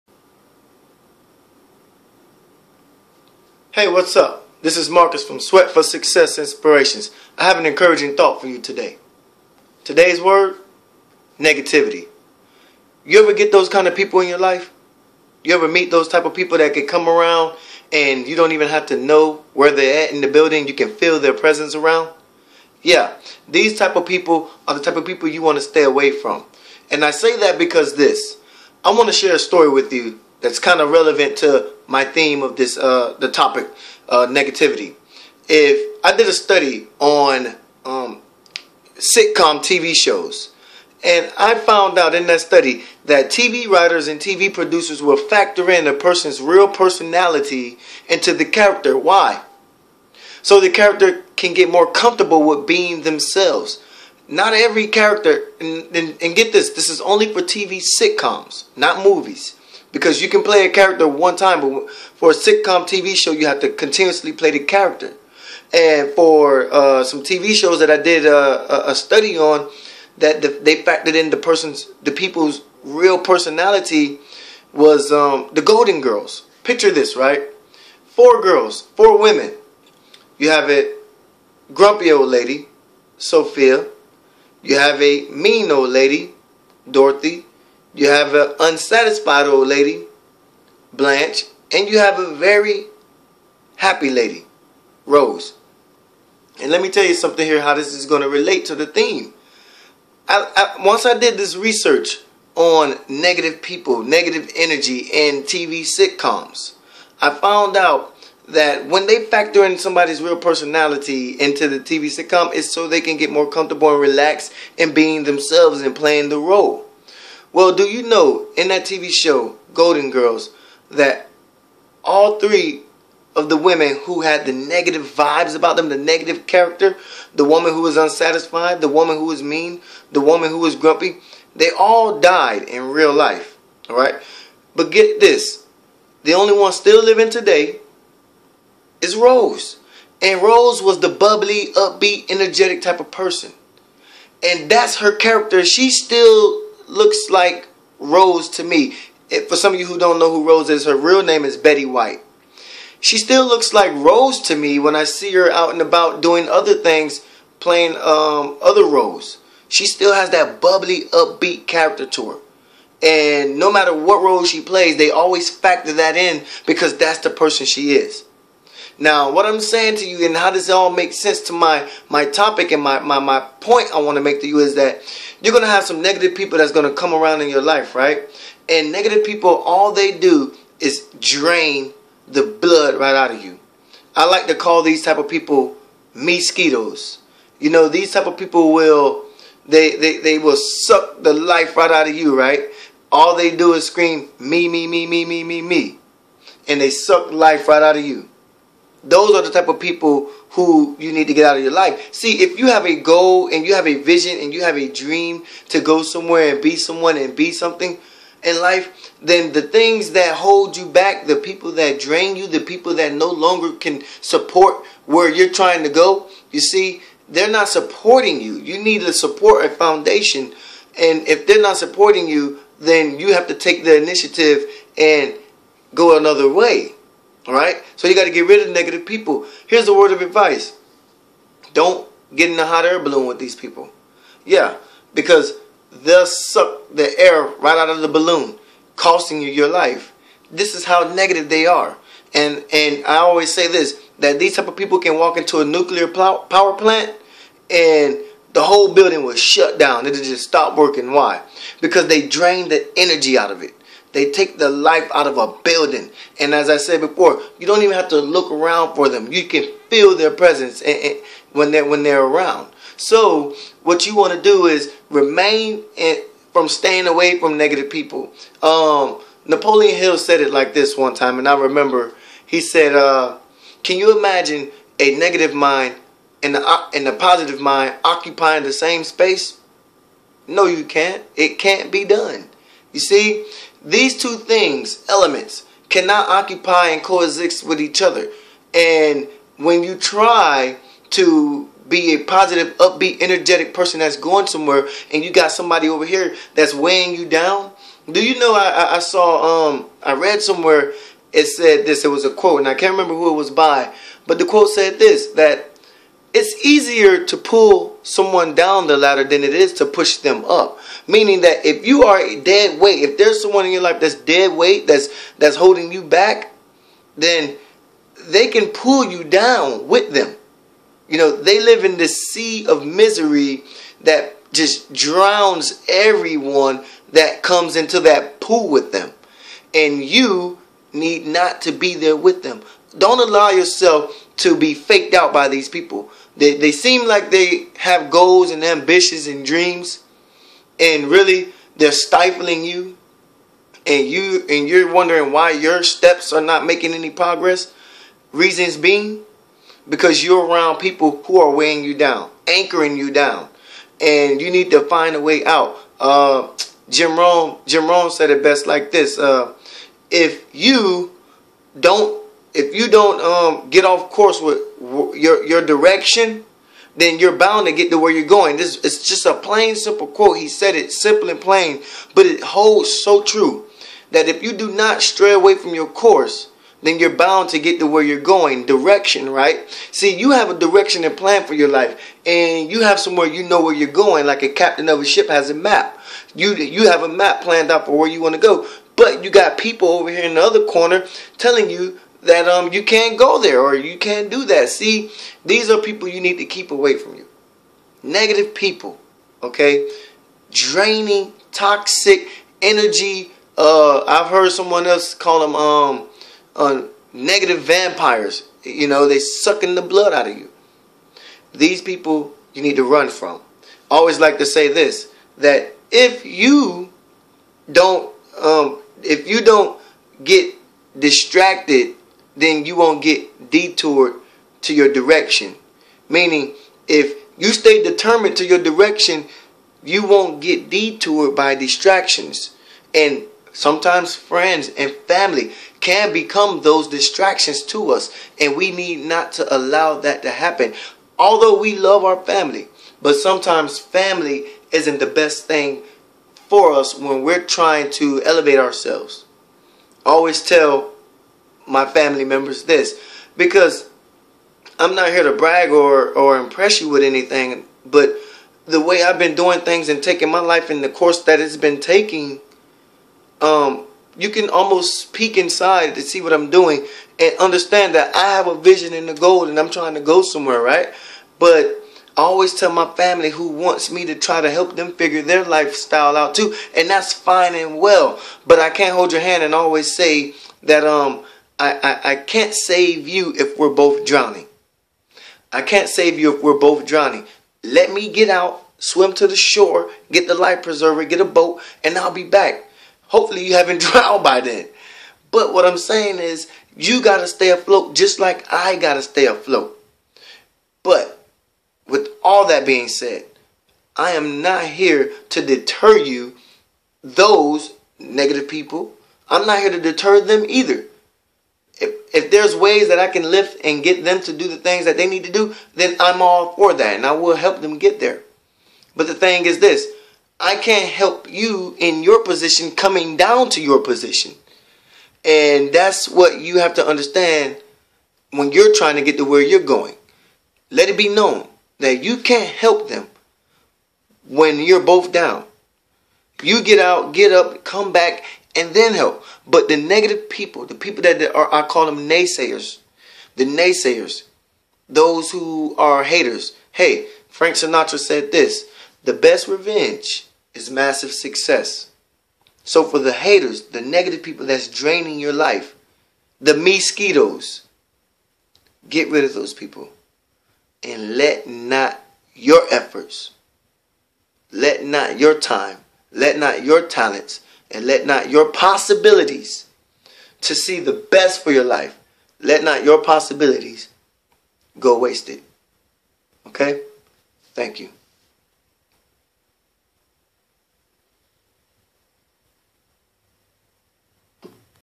SWET Inspirational Moment- How to deal with negativity people
inspirational moment for today-  is getting rid of negative people in your life... otherwise known as me-squitos... you know the types, all you hear them saying is me me me me me, while sucking the lifes blood out of you.